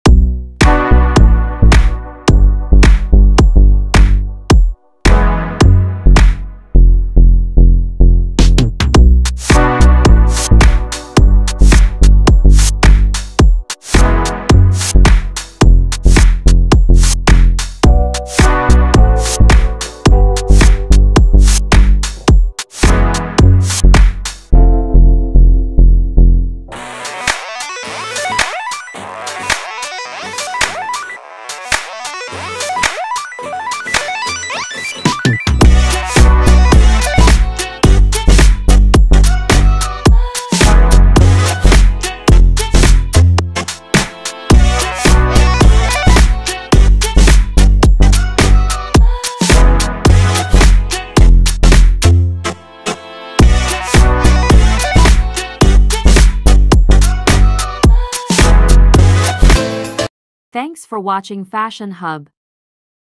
Stunning, gorgeous Jumka designs
This video is all about the latest, trendy and stylish jumka designs for girls.

In this channel you can watch all the latest fashion trends.

Comment what type of fashion you want to watch.